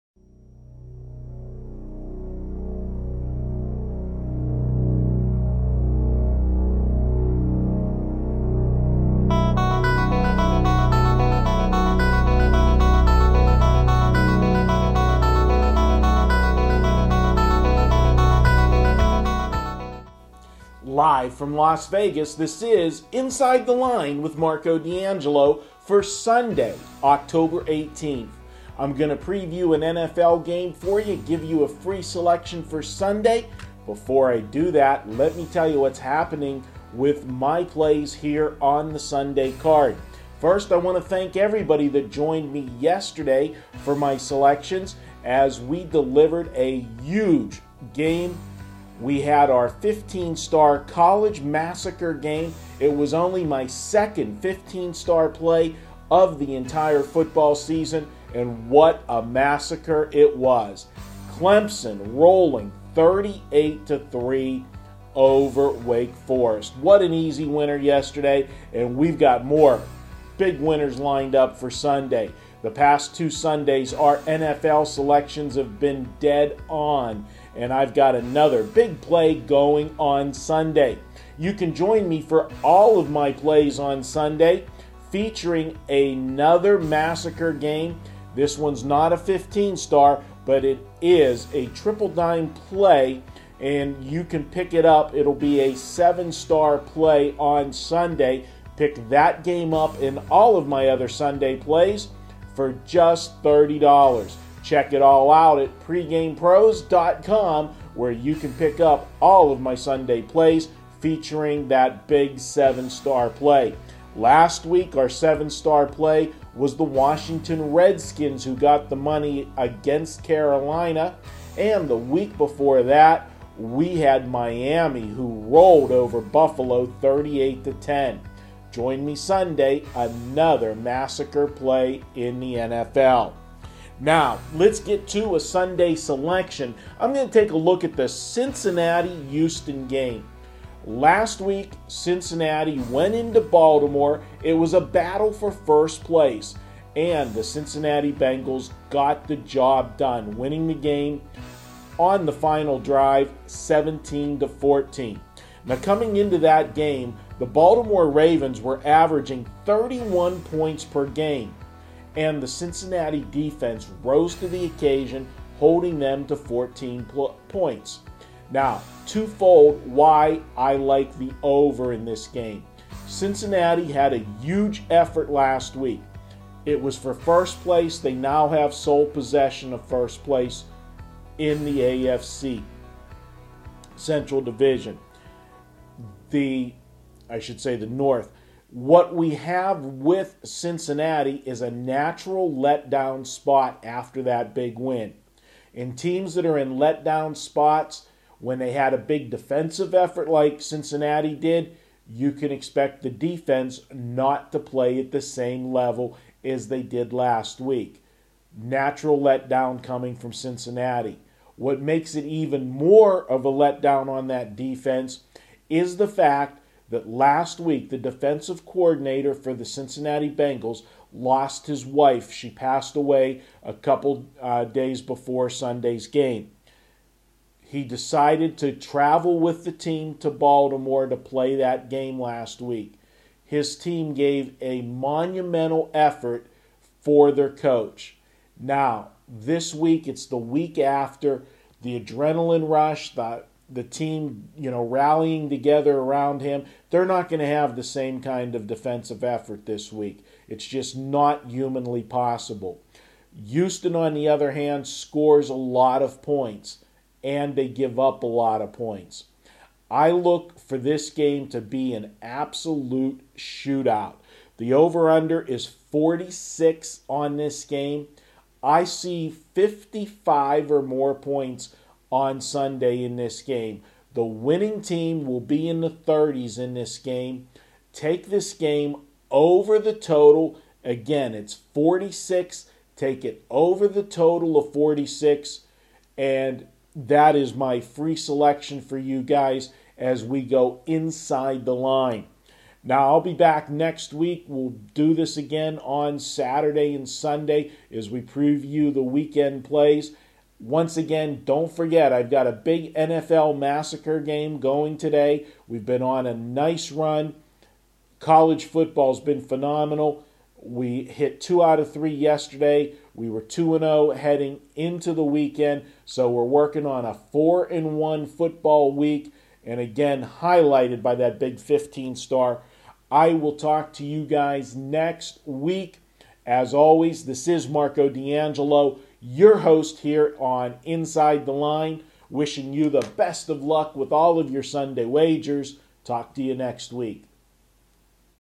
Inside the Line with Marco D'Angelo Oct 18
Las Vegas Betting Expert Marco D'Angelo gives Free Picks on Houston Texans @ Cincinnati Bengals Game for Sunday October 18, 2009.